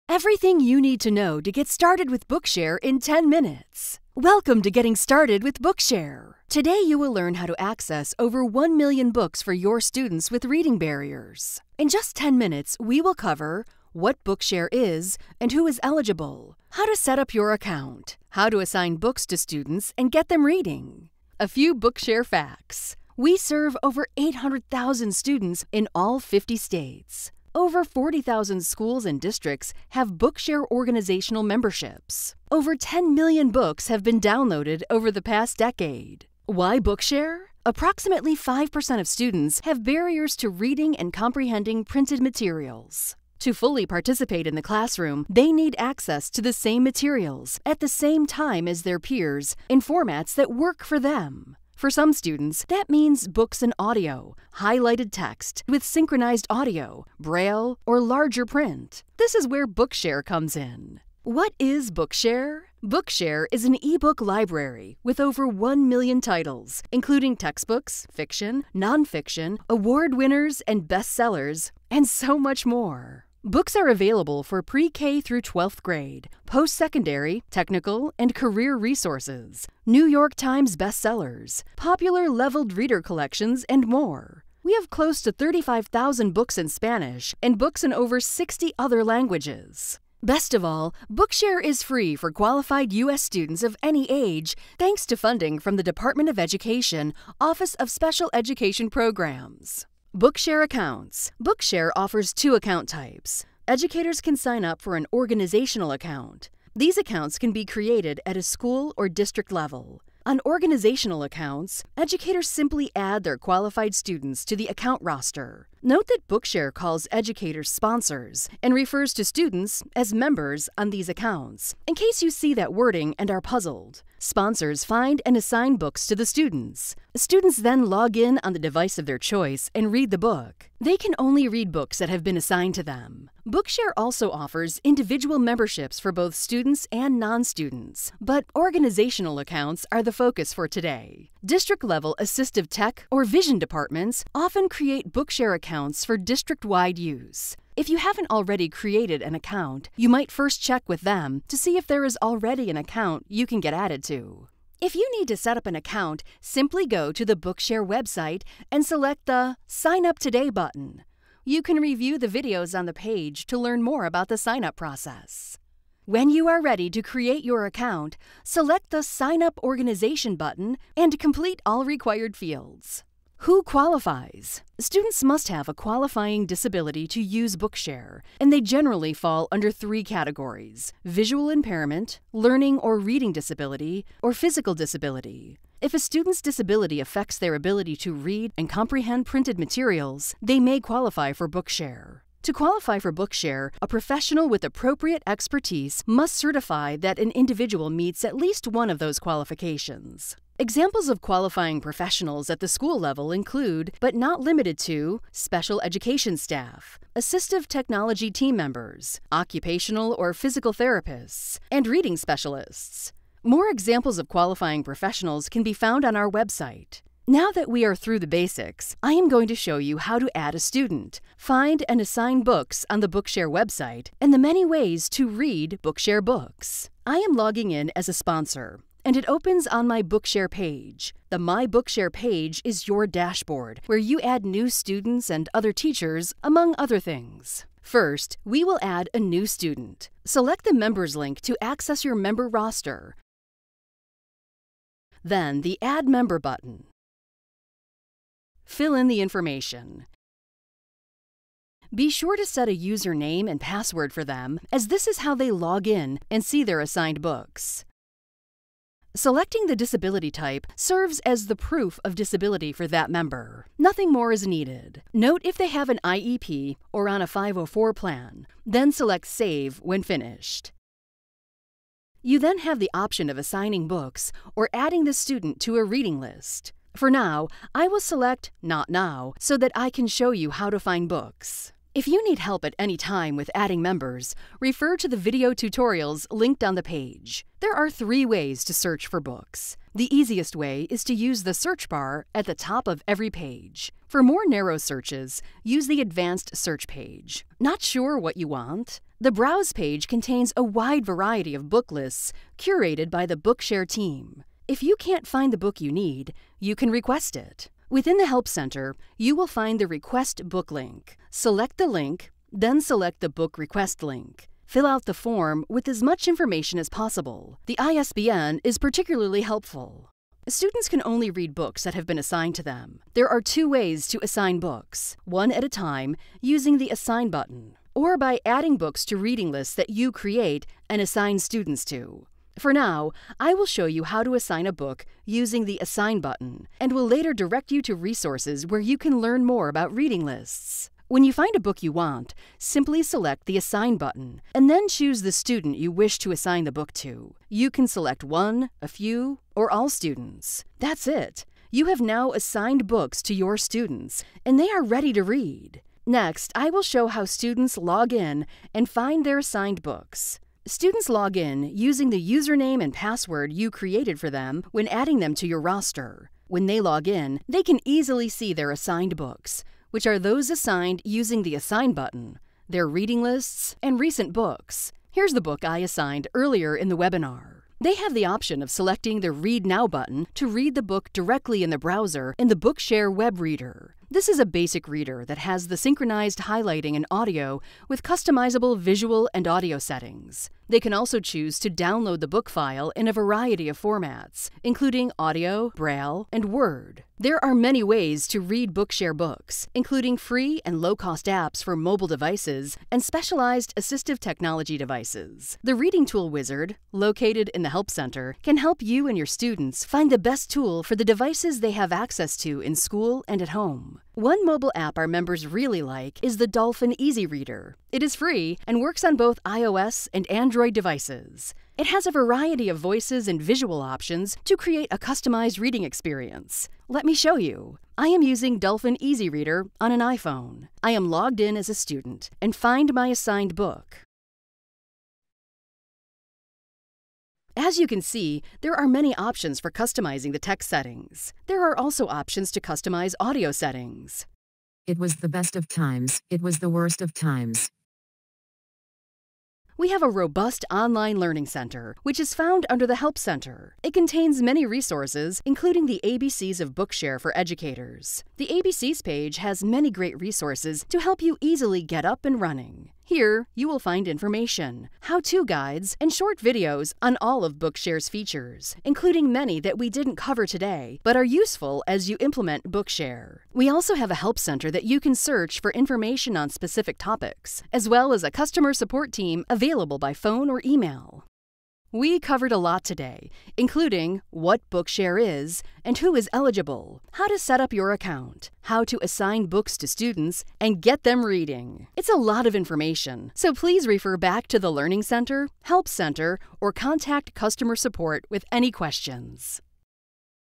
Educators Get Started with Bookshare in Just 10 Minutes!
Everything you need to know to get started with Bookshare in just 10 minutes including what Bookshare is, who is eligible, how to sign up and set up your account and get students reading!